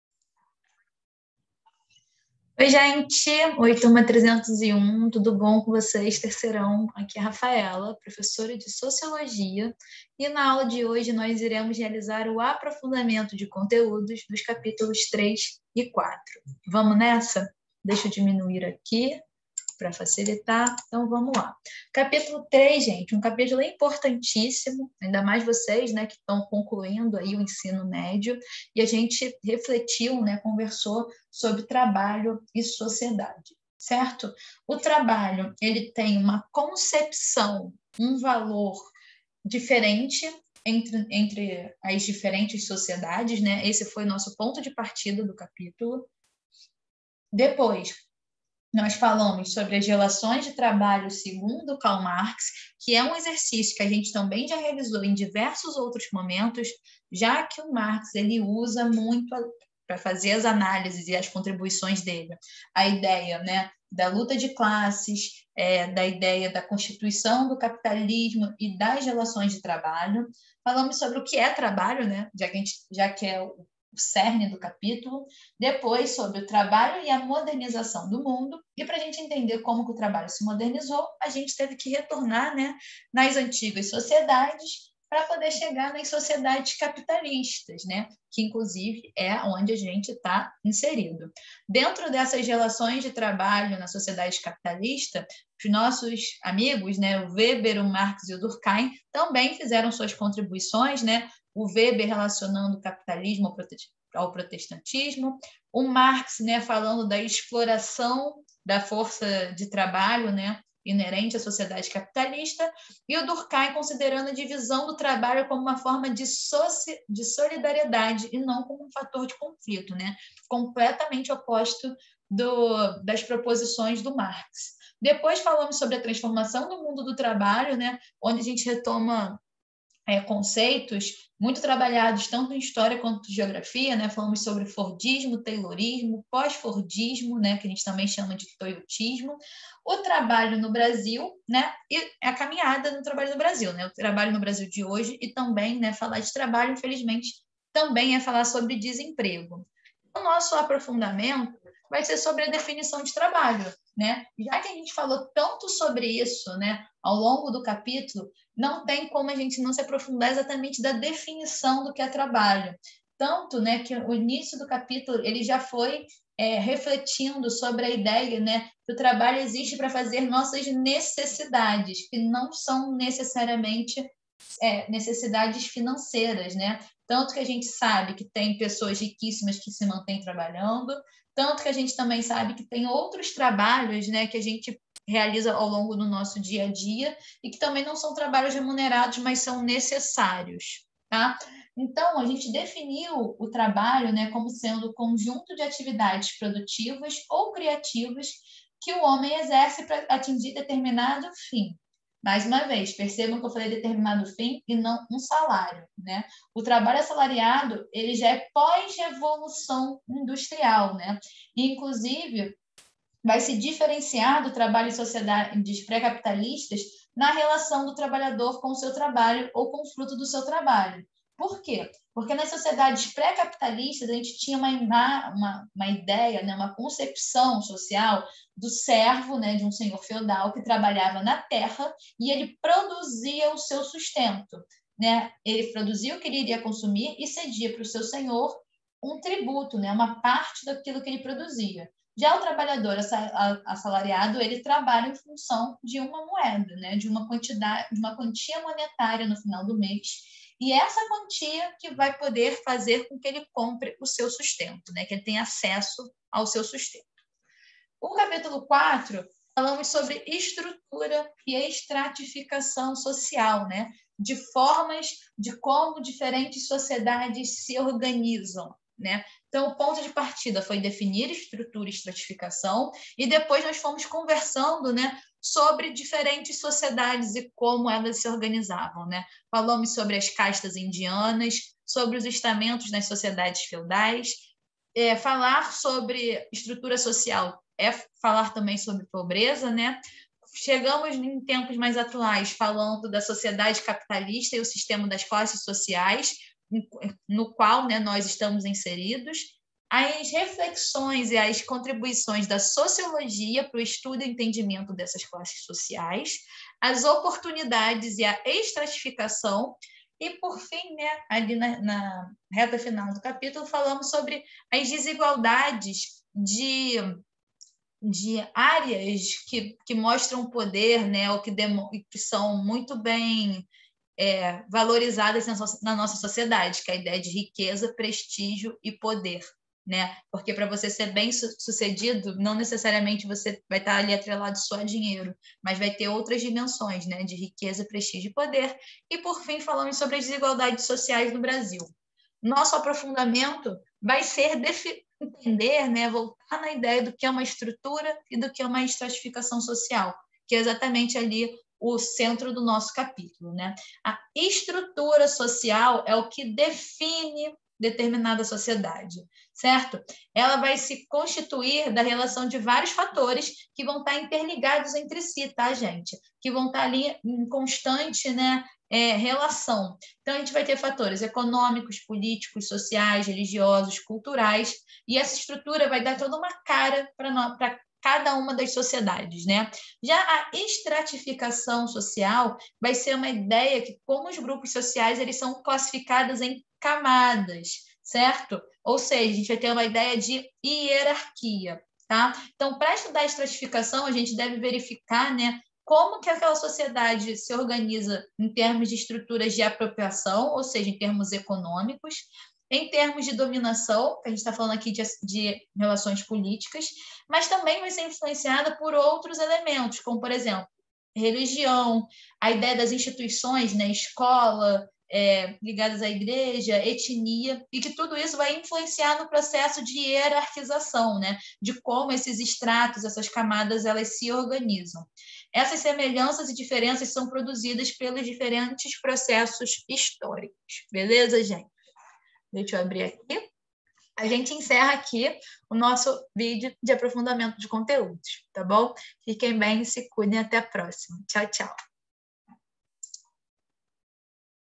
Sociologia 3ª Série Ensino Médio (Aula II - Aprofundamento de Conteúdos Caps III e IV)
Aula II - Aprofundamento de Conteúdos Caps III e IV
Professora Rafaela